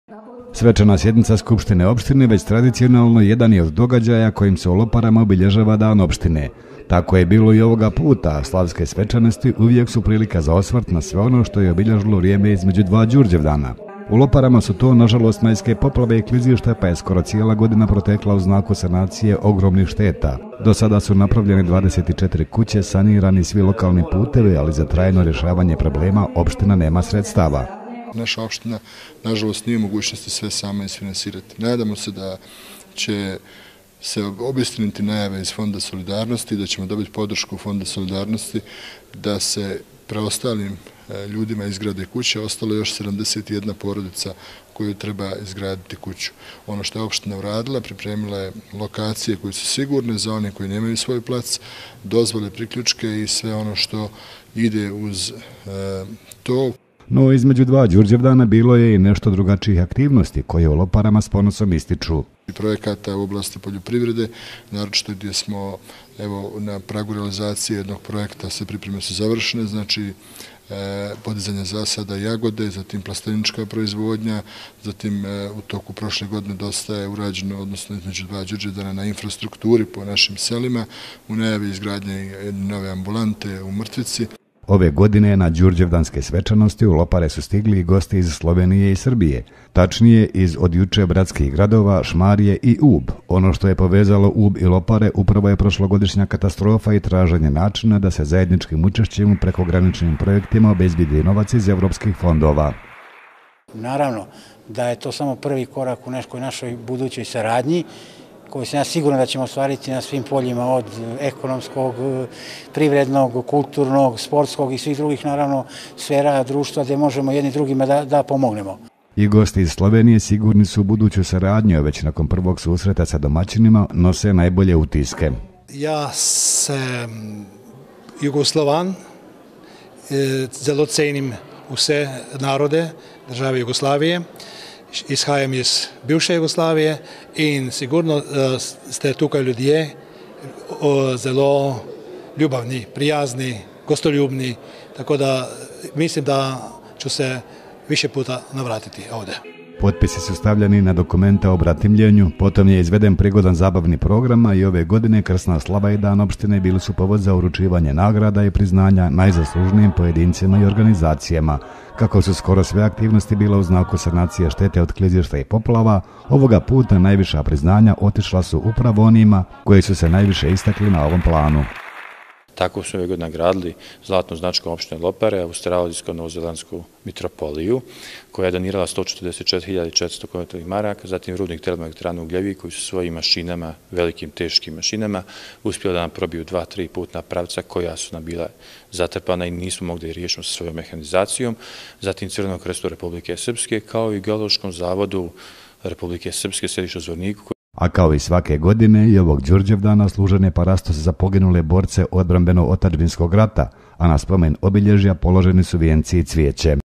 Lopare - Đurđevdan - Krsna slava opštine (rtv Hit)
Đurđevdan - Krsna slava opštine ( prilog rtv Hit)
Lopare 6. maj 2015. godine